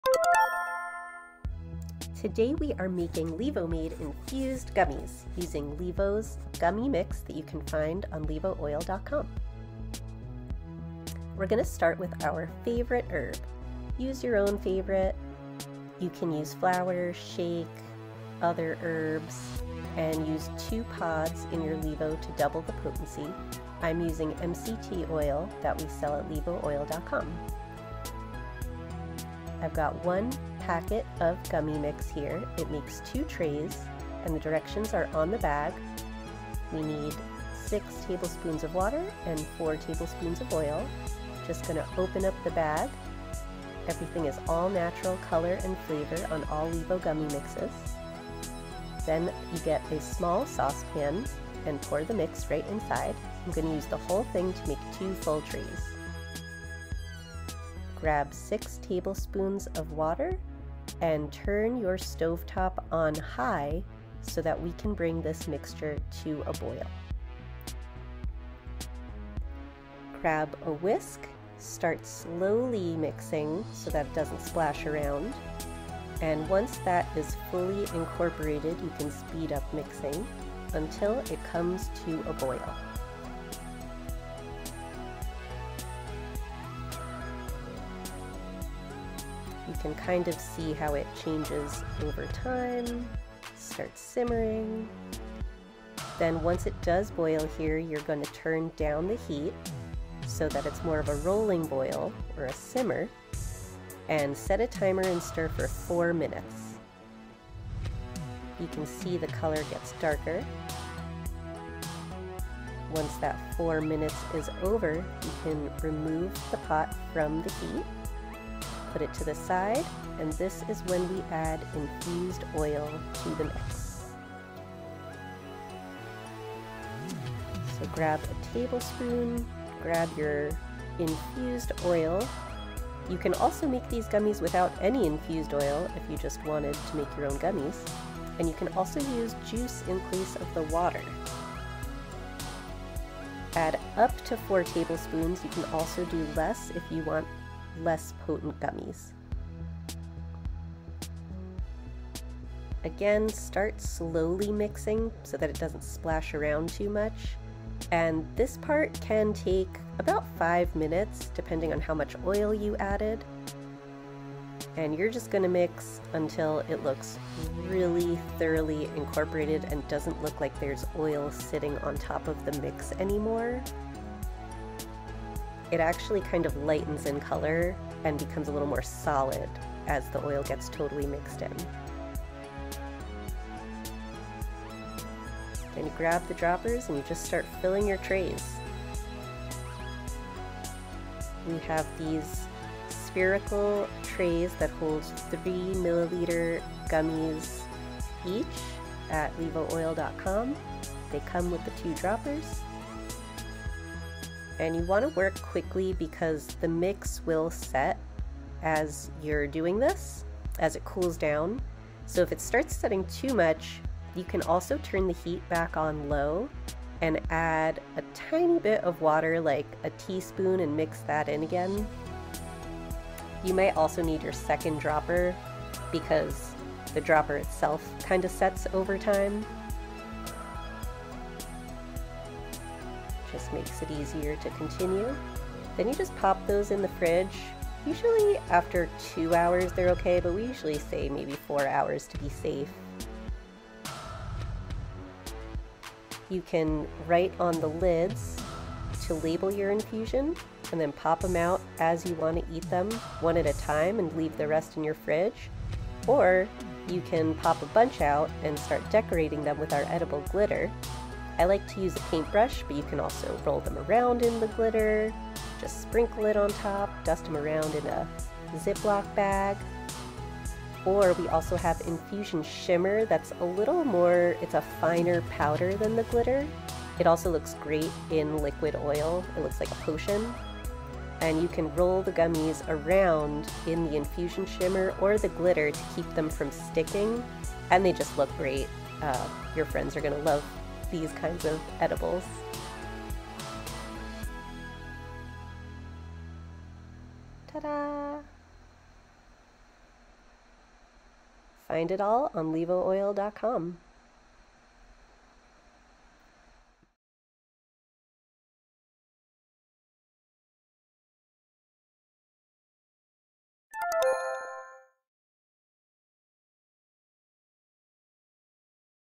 LĒVO Gummy Mix Instructions: How To Make Infused Gummies
Want to make infused gummies from scratch? LĒVO Gummy Mix is the easiest way to do it! Just add your favorite infused oil and water. In this video, we walk you through how to make our newest flavor: Strawberry Lemonade. Follow these LĒVO Gummy Mix instructions for a failproof way to make infused gummies at home.

Share your LĒVO creations with us on social media by using LEVOmade and find us on all major platforms with the tags below